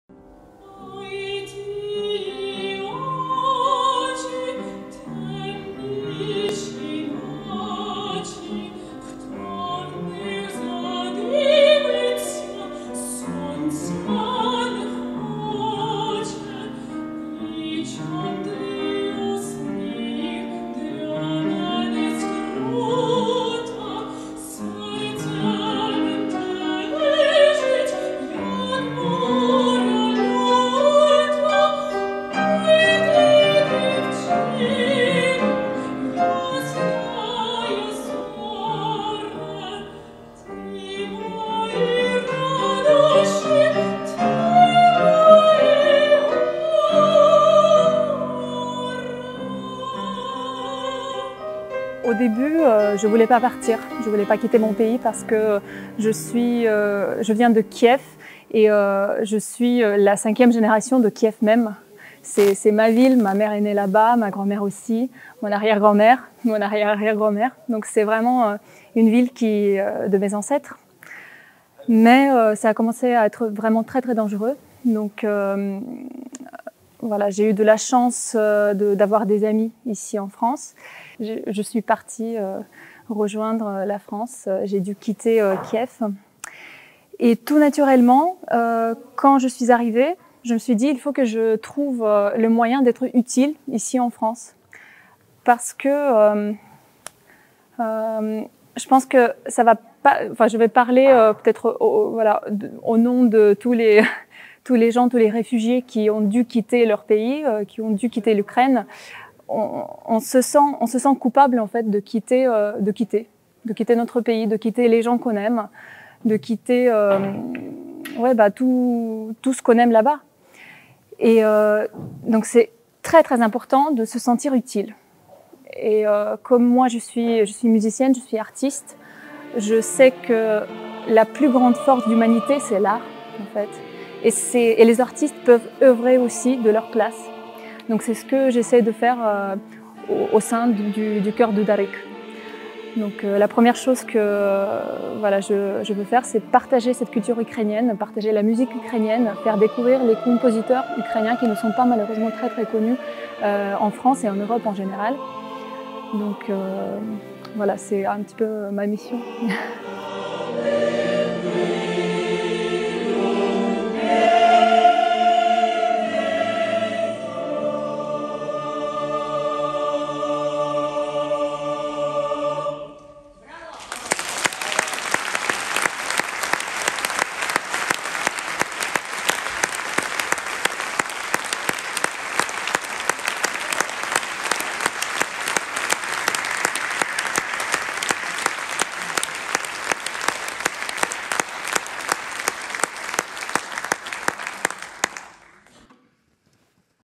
Victoria Didenko, réfugiée et soliste du Chœur ukrainien de Lyon Doudaryk
Lorsque les Russes ont lancé les premières bombes sur la capitale ukrainienne, Victoria Didenko était là-bas avec toute sa famille. Elle a vécu 8 ans en France et au mois de janvier 2022 est rentrée à Kyiv, sa ville natale pour continuer ses études artistiques. Chanteuse lyrique et chef de chœur, Victoria est de retour en France, recemment elle s'est rejoint le Chœur ukrainien de Lyon Doudaryk .
Vous avez pu apprecié sa voix le 9 avril 2022 lors du concert caritatif de Doudaryk au Sémaphore - Théâtre d'Irigny , avec la participation de musiciens de l'École de Musique d'Irigny.
Vidéo réalisée par Iurii Panin (PIN Production) en collaboration avec Markiian Peretiatko  Plus d'info : lyonukraine.org